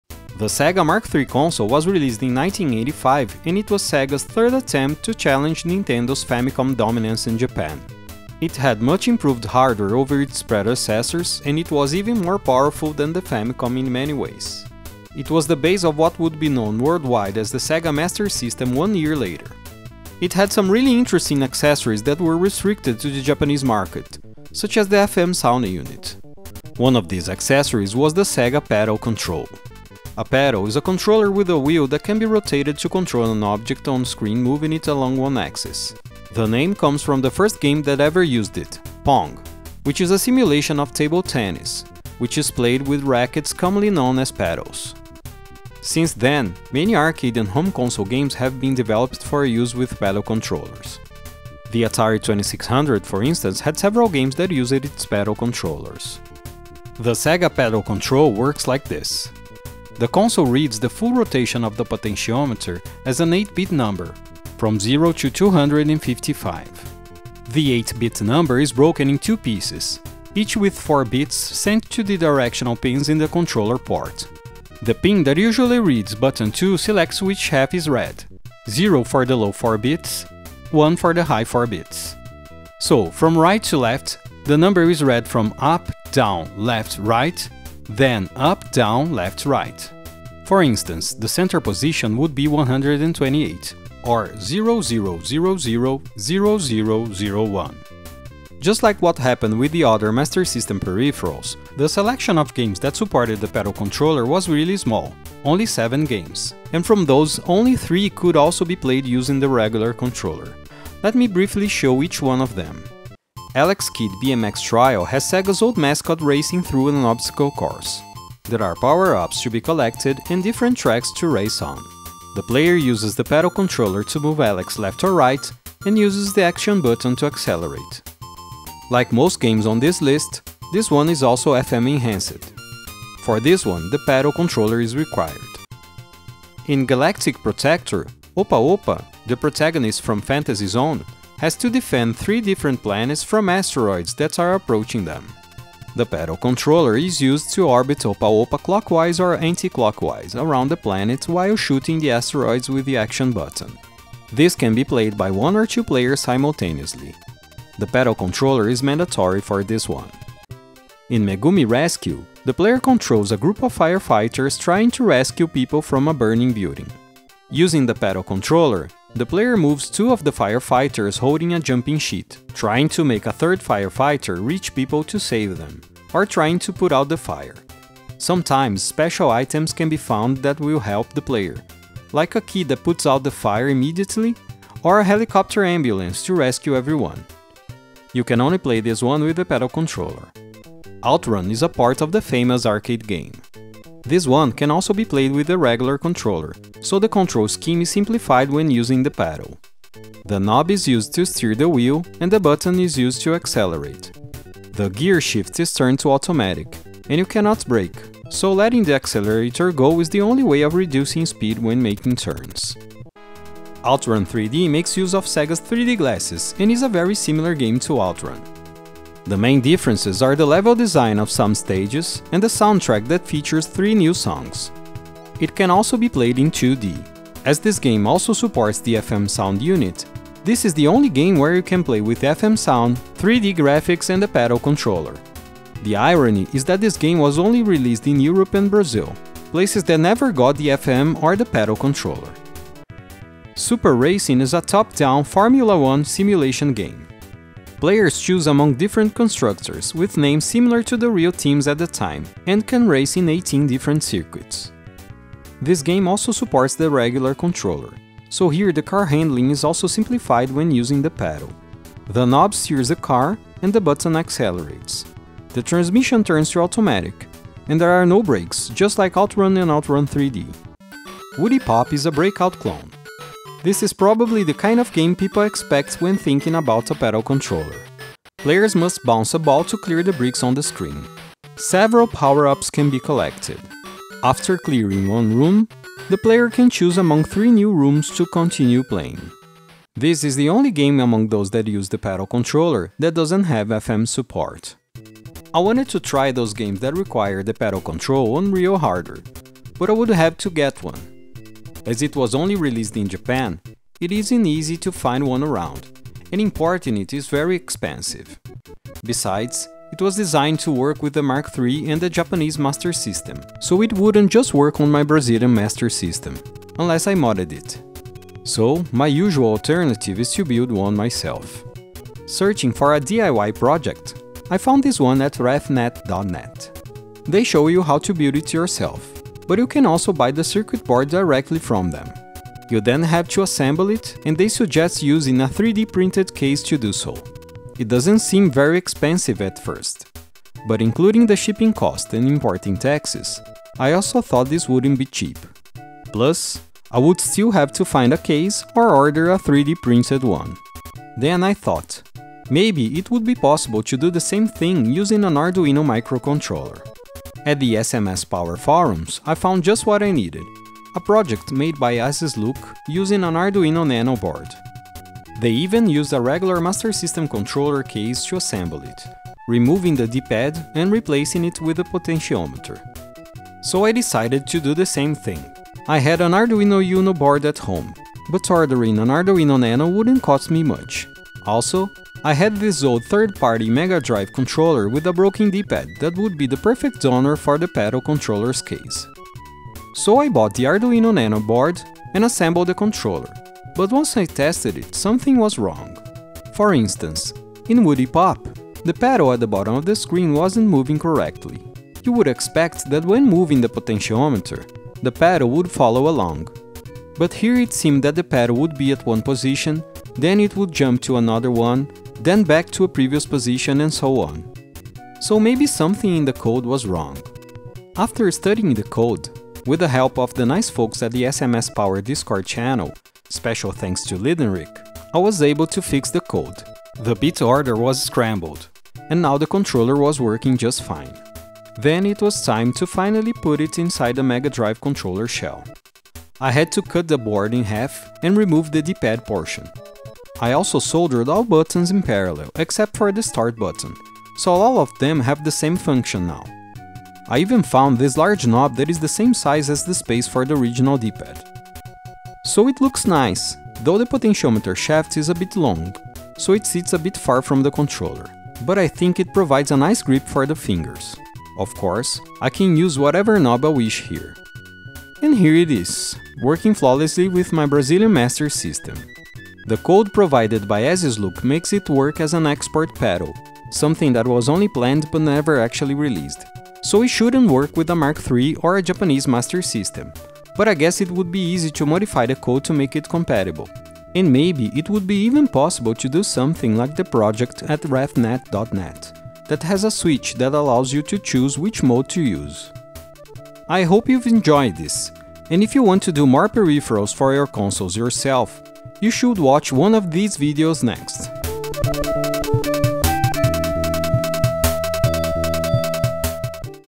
The RAREST Master System controller (and how I built one myself)!
In this video I present the SEGA Paddle Control, its supported games, and discuss how you can build your own paddle controller for the SEGA Master System using an Arduino Nano board.